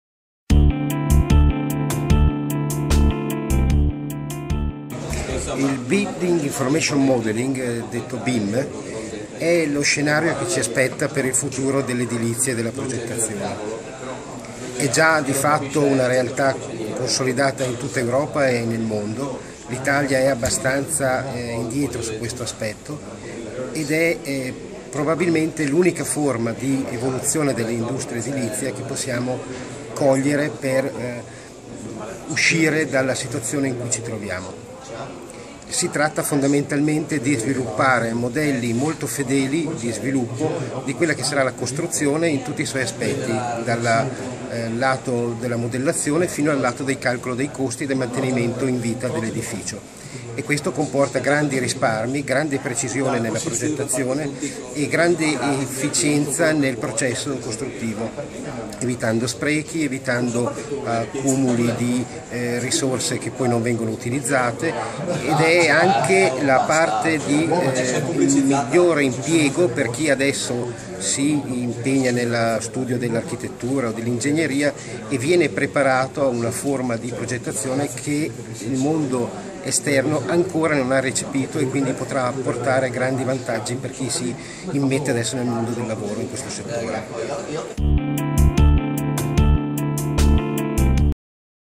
FF2014 Vittorio Frego
ECOSISTEMA 3D

Le tecnologie digitali sono pervasive e evolvono con una rapidità incredibile.
Per questo viene rinnovato l’ appuntamento con alcuni dei protagonisti delle tecnologie 3D e del digitale, per fare il punto della situazione e tracciare i possibili percorsi di sviluppo come li vede ed immagina chi opera con le grandi aziende del settore.

VITTORIO FREGO BIM Solution Expert